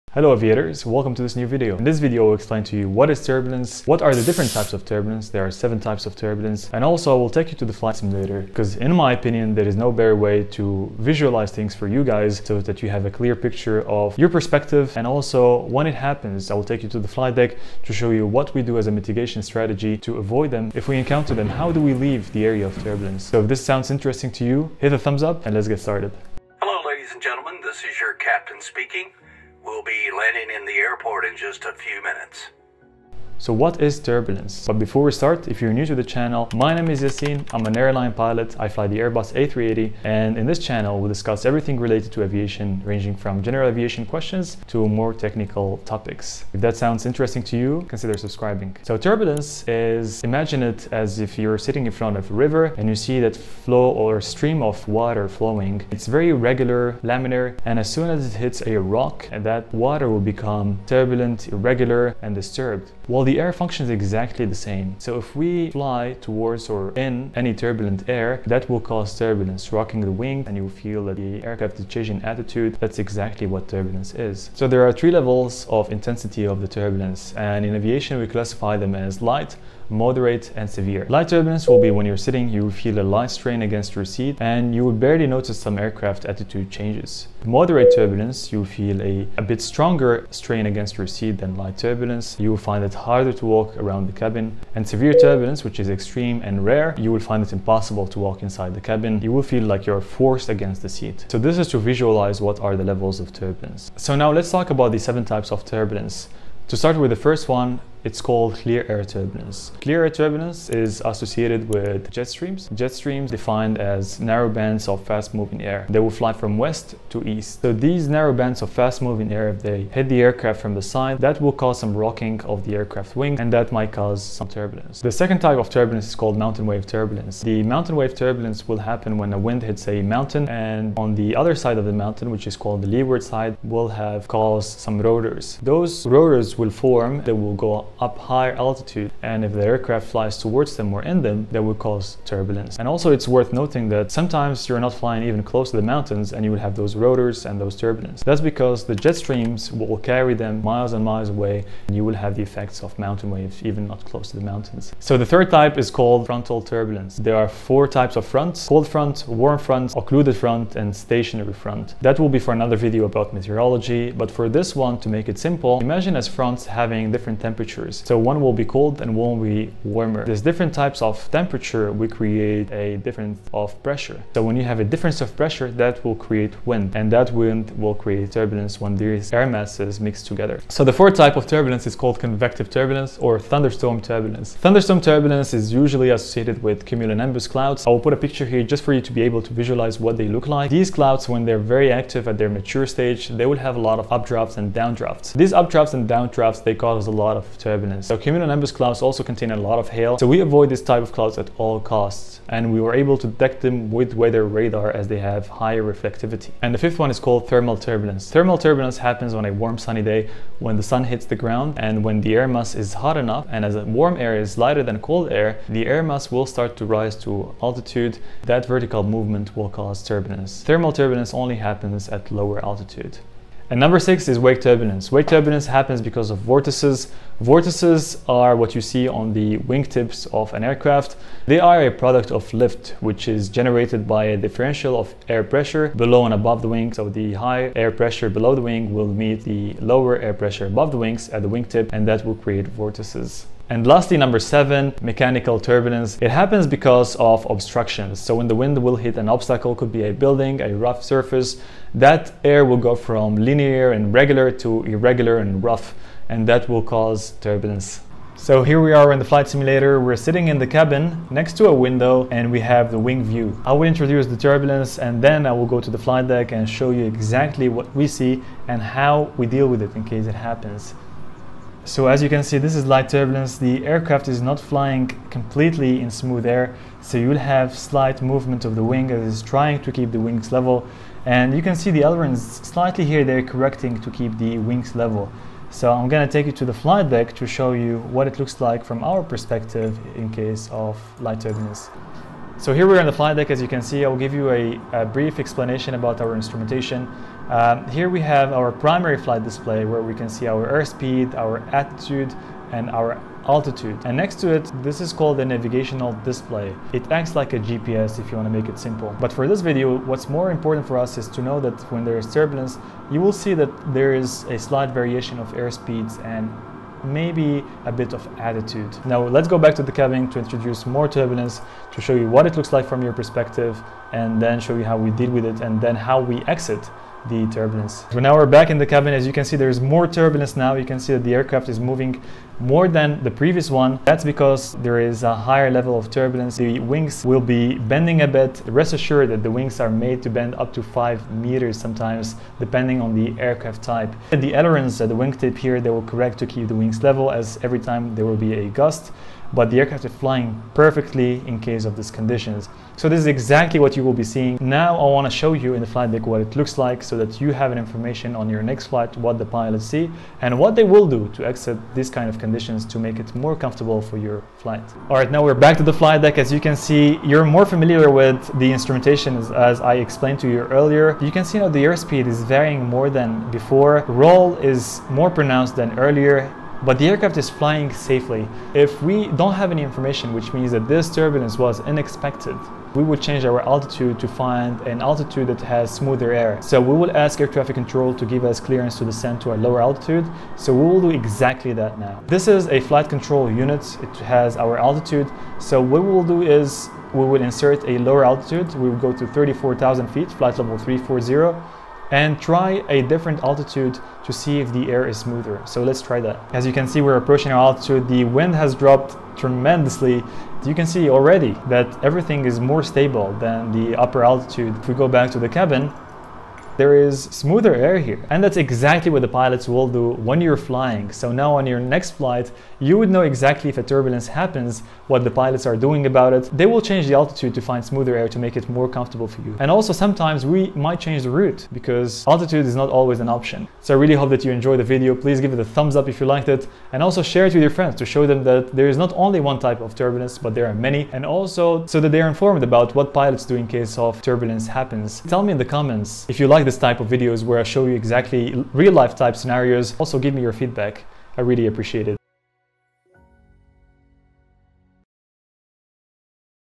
Turbulence Demystified | Insights from an AIRLINE PILOT
Hello Friends,
In this video I will take you through the 7 types of turbulence.
You will understand the difference between the different levels of turbulence.
And Finally, we will use flight simulator to simulate real life scenario, to visualize the event.

Timestamps :
0:00 Intro
0:25 Definition of Turbulence
1:16 Levels of Turbulence
1:49 CLEAR AIR TURBULENCE
2:07 MOUNTAIN WAVE TURBULENCE
2:37 FRONTAL TURBULENCE
3:00 CONVECTIVE TURBULENCE
3:28 THERMAL TURBULENCE
3:45 WAKE TURBULENCE
4:06 MECHANICAL TURBULENCE
4:18 SIMULATOR
6:33 AVOIDING TURBULENCE
7:00 Outro

As Always, if you have more questions after watching this video, ask me in the comments below :)

Until the next one, stay safe.
Your Pilot Yassine